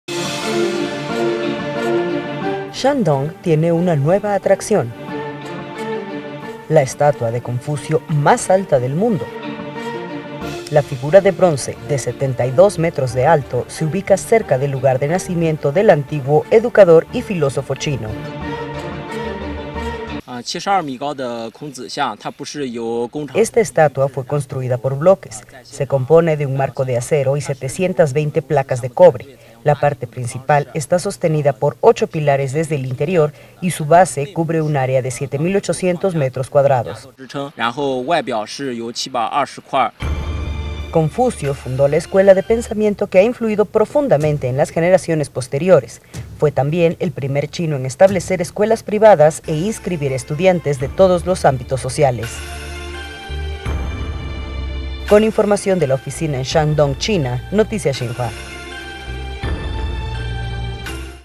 China revela la estatua de Confucio más alta del mundo
La figura de bronce de 72 metros de alto se ubica cerca del lugar de nacimiento del antiguo educador y filósofo chino.